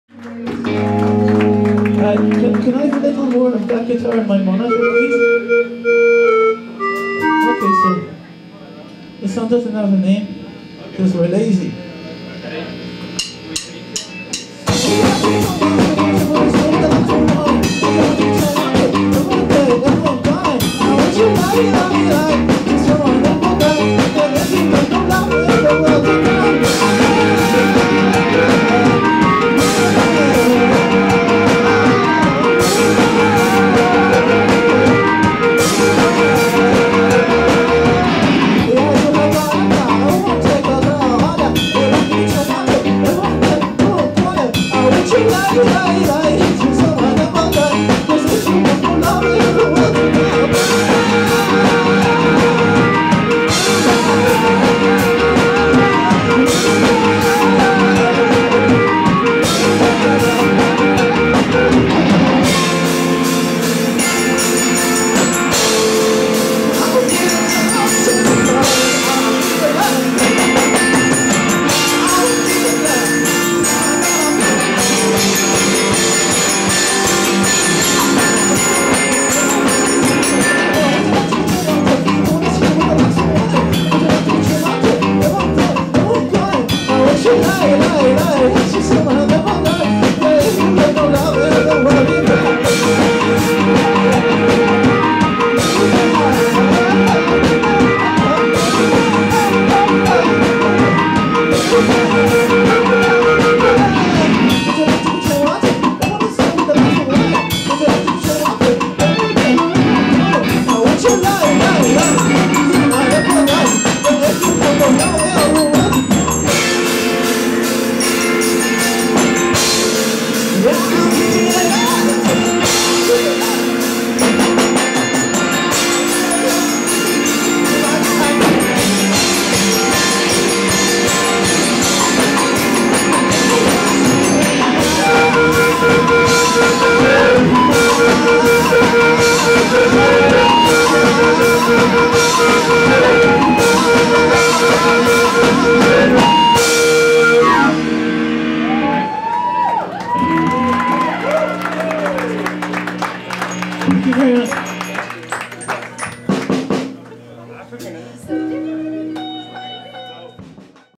grand pocket orchestra plays Toronto CMW 09
Irish indy band grand pocket orchestra plays at The Hideout in Toronto during Canadian Music Week on
 Thursday, March 12, 2009. Part of the Music from Ireland showcase, the band is: "P and P and B and Flesh".

The video was shot in The Hideout, a club in Toronto with hideous lighting (no spots & messed up LED
overhead lights that flicker at an annoying rate.) More evidence of sadistic club owners who enjoy making
people suffer for their music. This just means avoiding The Hidehout in the future, though.

Video by Henry Martinuk, Chernozym Video